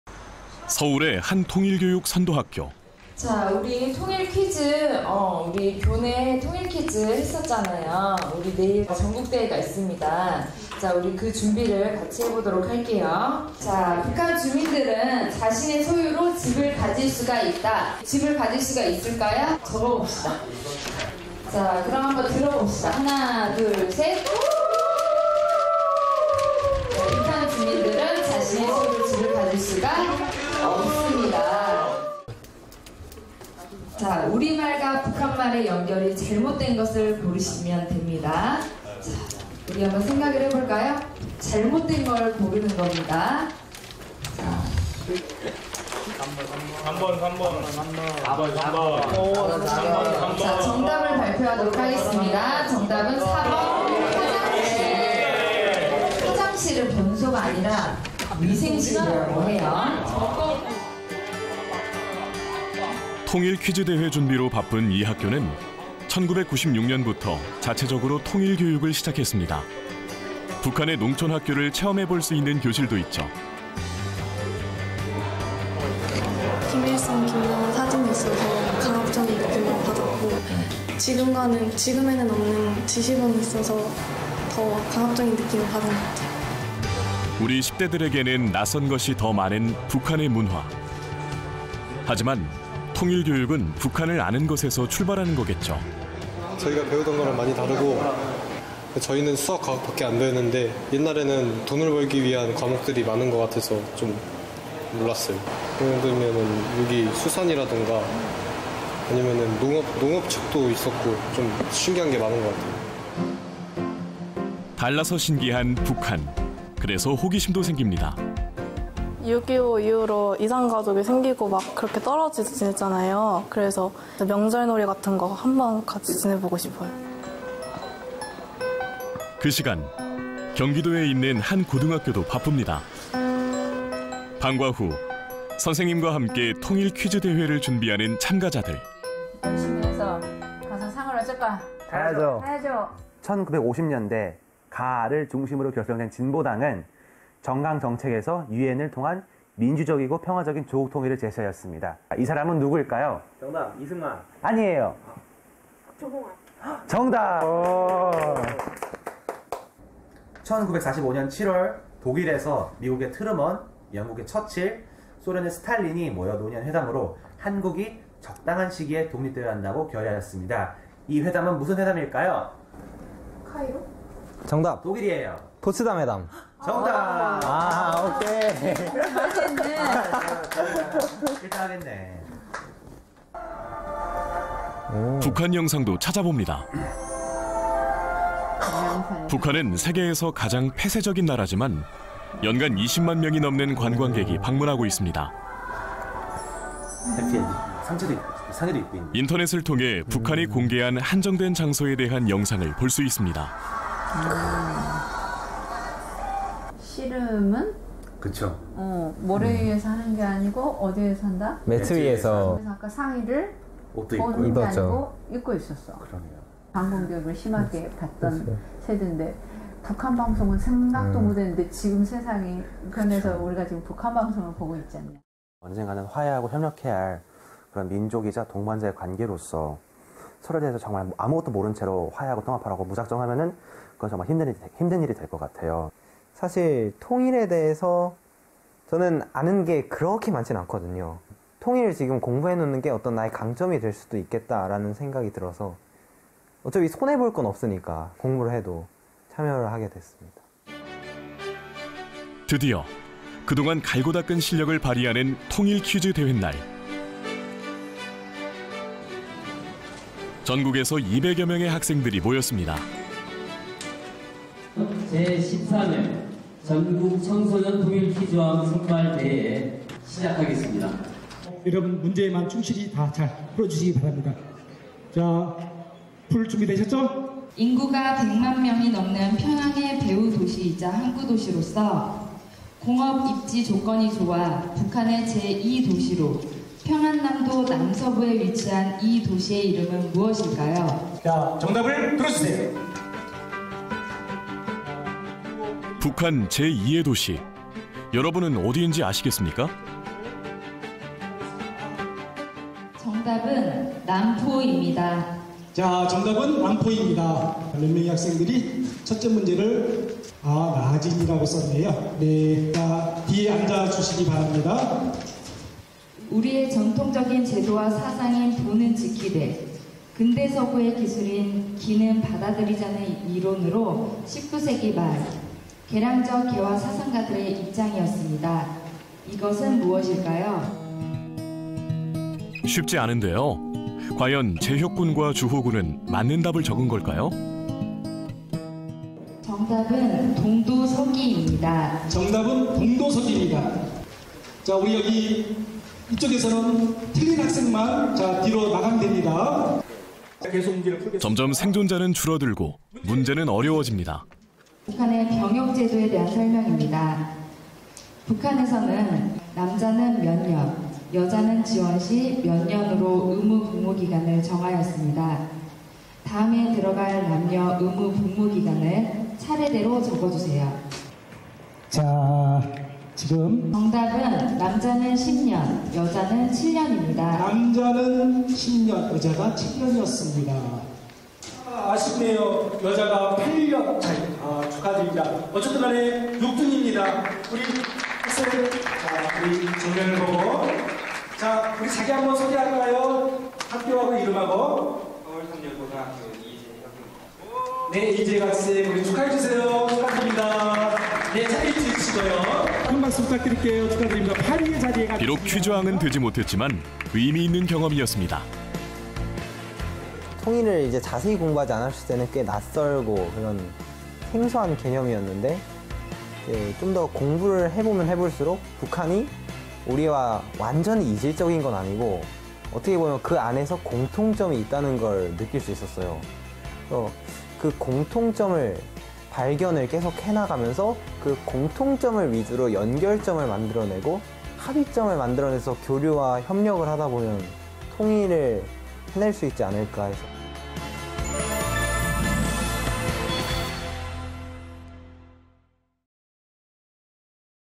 진짜 통일퀴즈왕을 찾아! 울려라, 통일골든벨-!
낯선 것이 더 많은 북한의 문화지만
통일교육은 북한을 아는 것에서부터 출발하는 것!
북한 주민들은 자신의 소유로 집을 가질 수 있다?
남한말과 북한말이 올바르게 짝지어진 것은?
📢여러분도 영상에서 함께 문제를 풀어보세요😉
🔔통일퀴즈대회를 준비하며 학생들은
다르면서도 그 속의 공통점을 발견해 나가는
시간을 가졌습니다!
자세히 공부하지 않았을 때는
낯설고 생소했던 통일이
이제는 학생들에게
조금 더 가까운 이야기가 되었으면 합니다🙆